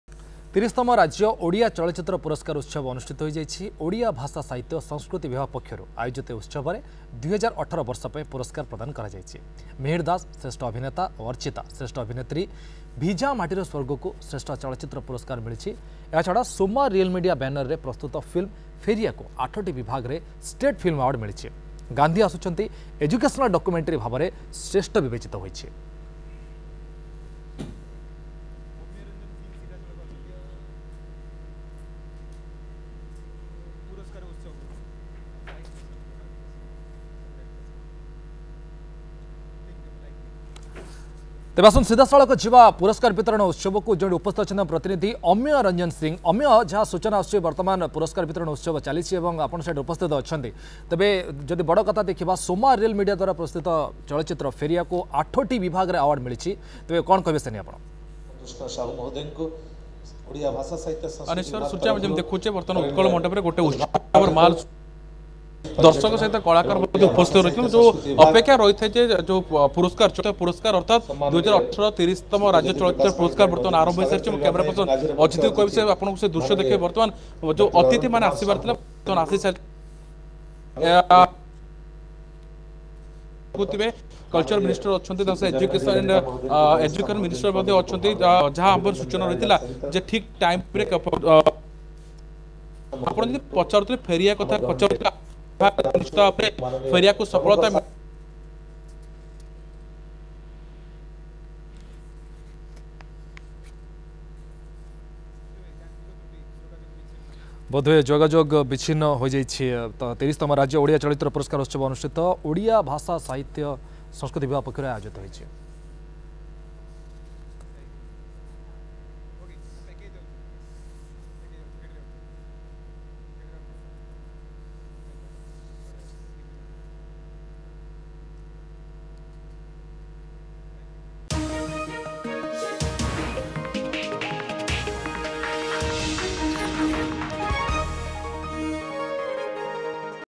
30th Odisha State Film Awards 2019 Festival Organised In Bhubaneswar
30th Odisha State Film Awards 2019 Festival Organised In Bhubaneswar